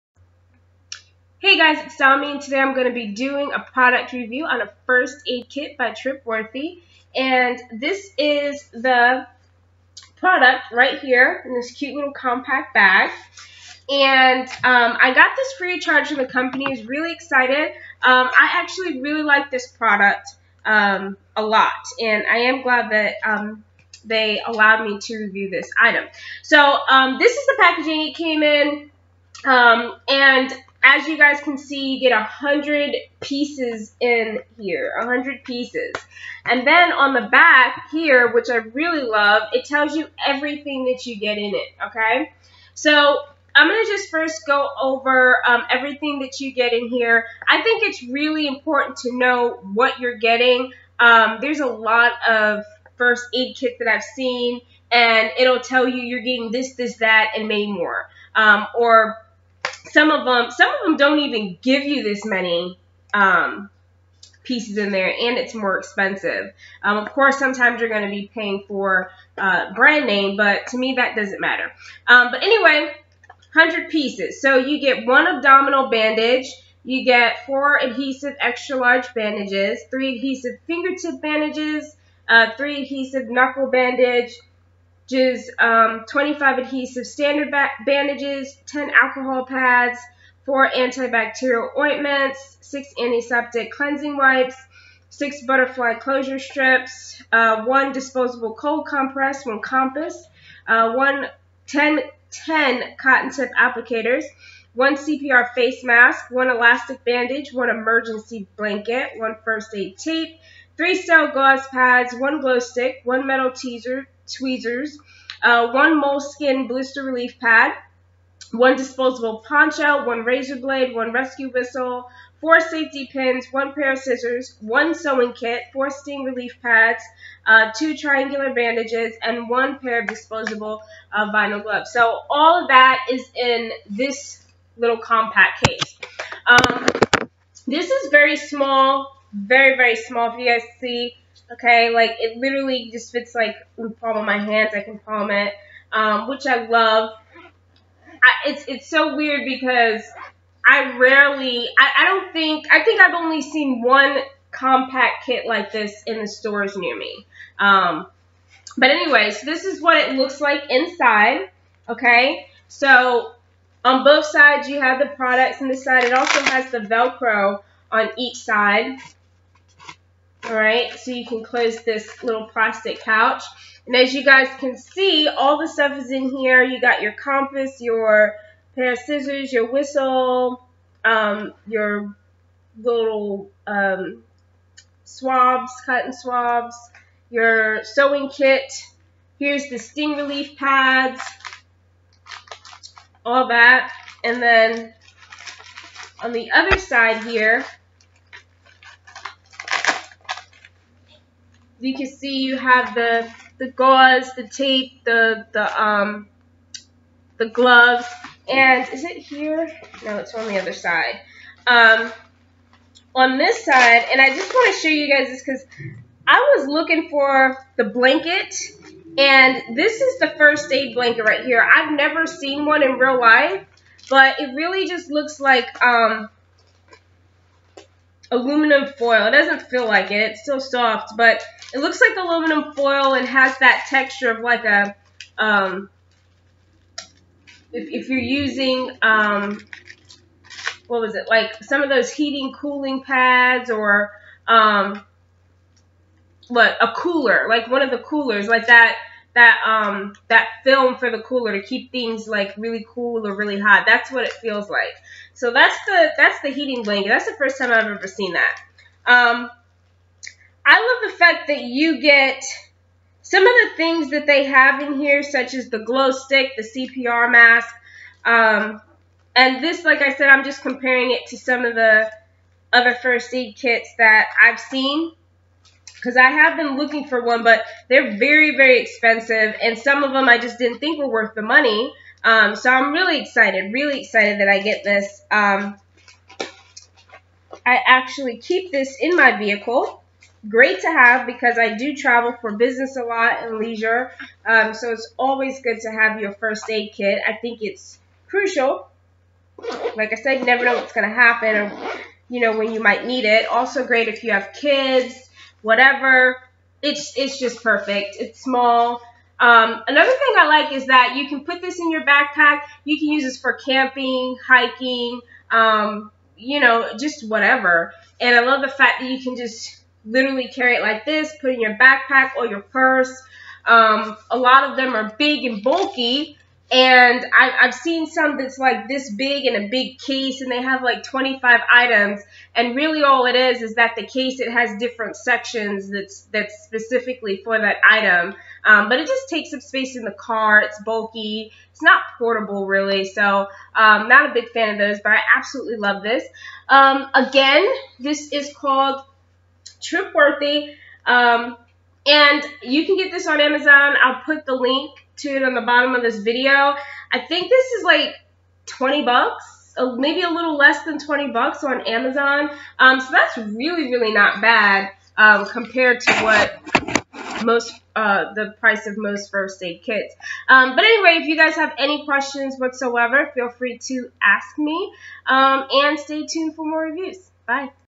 Product Review on TripWorth 100 piece compact first aid kit
* I received this product free of charge in exchange for my honest opinion*